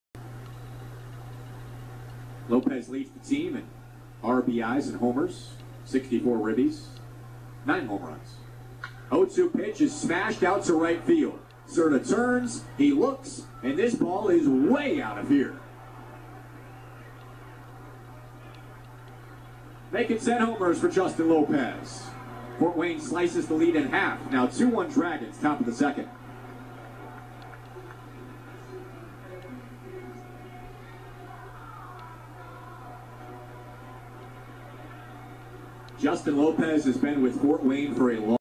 Ft. Wayne TinCaps' Justin Lopez hits home run vs. Dayton, 9/15/21
Ft. Wayne TinCaps' Justin Lopez hits a home run vs. Dayton, 9/15/21.